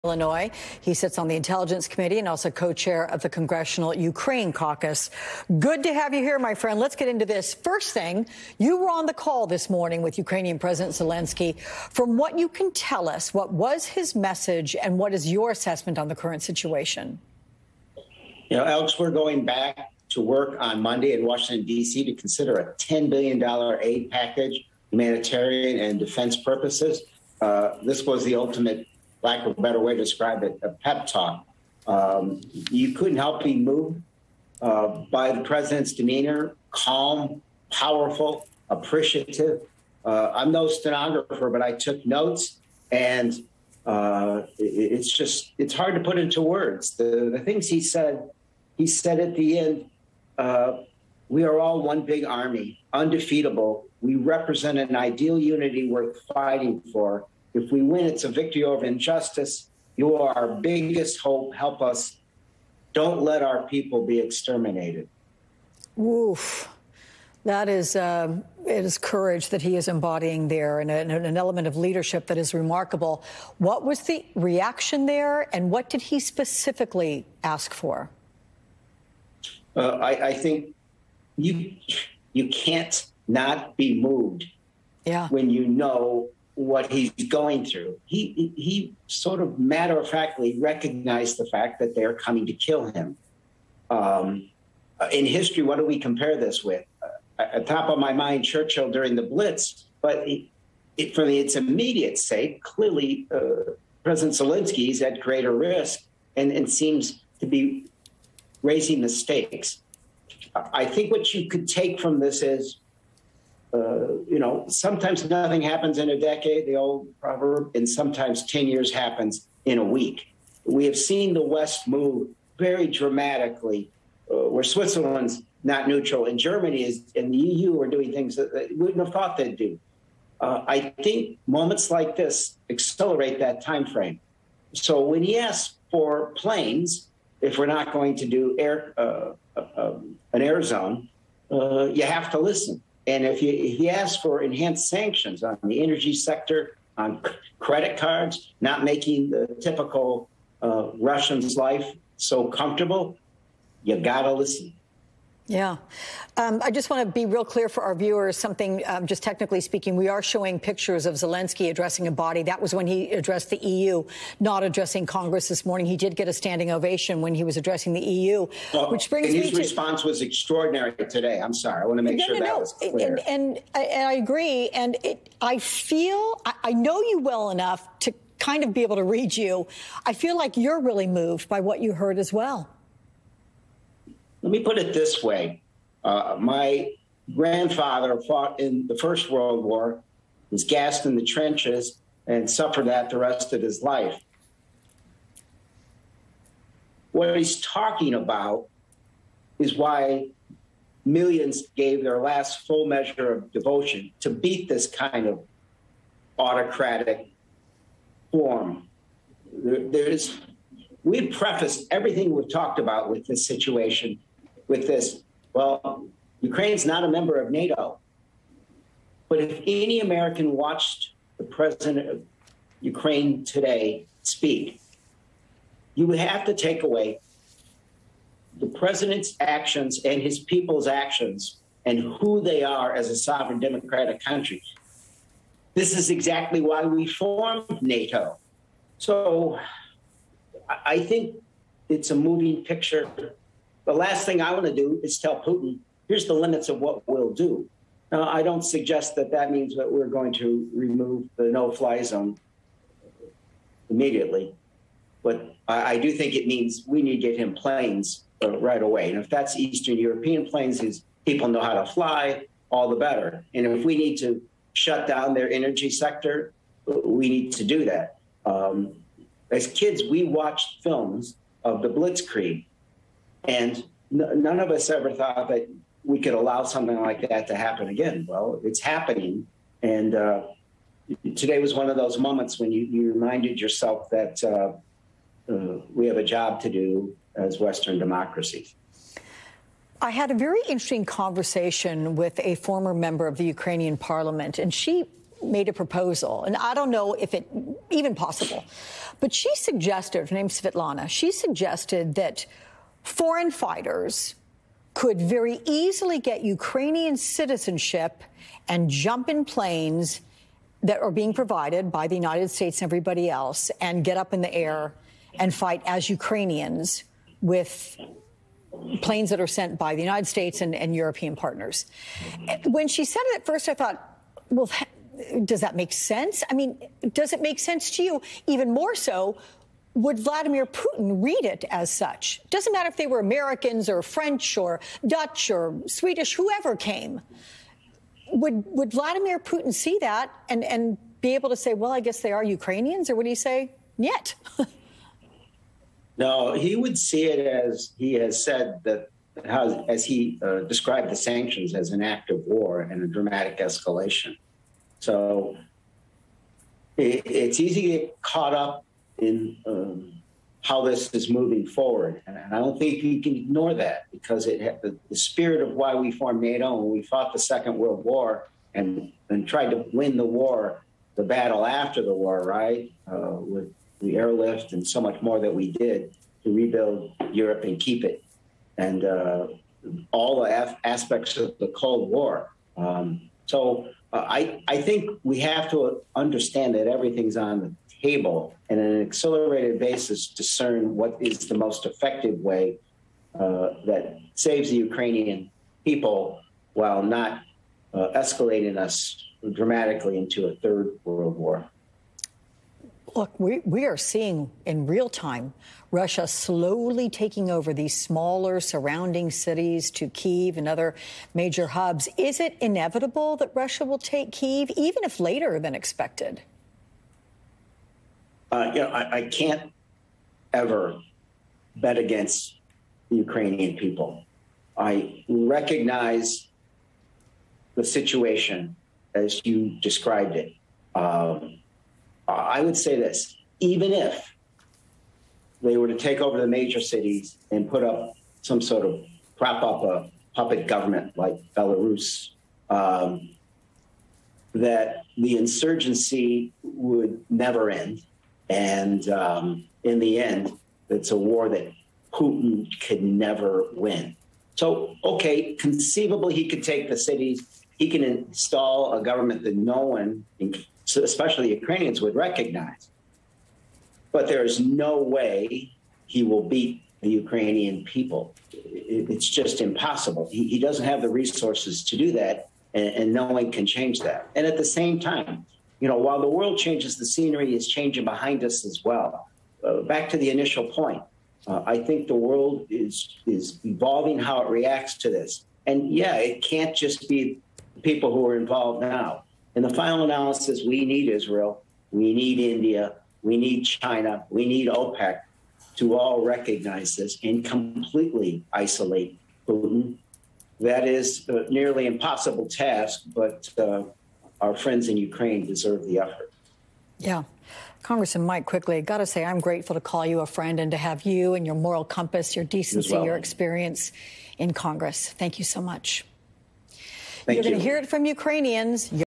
Quigley Recounts Call with Ukrainian President Zelenskyy
U.S. Representative Mike Quigley (IL-05), a Co-Chair of the Congressional Ukraine Caucus, spoke with MSNBC's Alex Witt after a virtual call with Ukrainian President Volodymyr Zelenskyy.